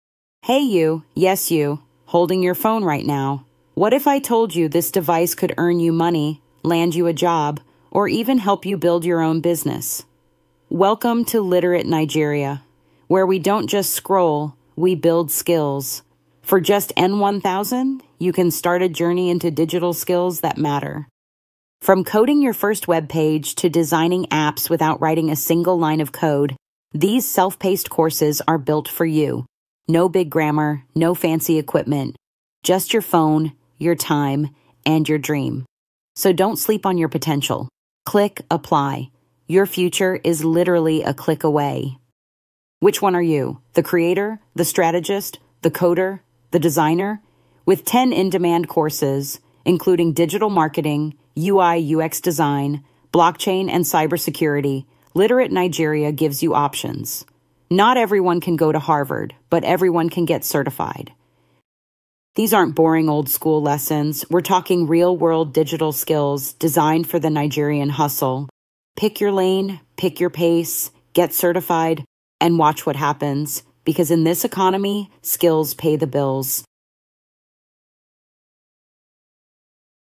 Your Phone Can Change Your Life – Literate Nigeria Digital Skills for Youth
Description: What if the device in your hand could unlock a better future?
Welcome to Literate Nigeria – a self-paced, low-cost digital skills platform built for young Nigerians. For just ₦1,000, gain access to beginner-friendly courses like Game Development, Web Design, and AI Tools, right from your phone.

No laptop? No problem.
No time? Learn at your pace.
No experience? We’ve got you.

This isn’t just training. It’s your ticket to earning more, building something real, and owning your digital future.

Join thousands already learning and winning.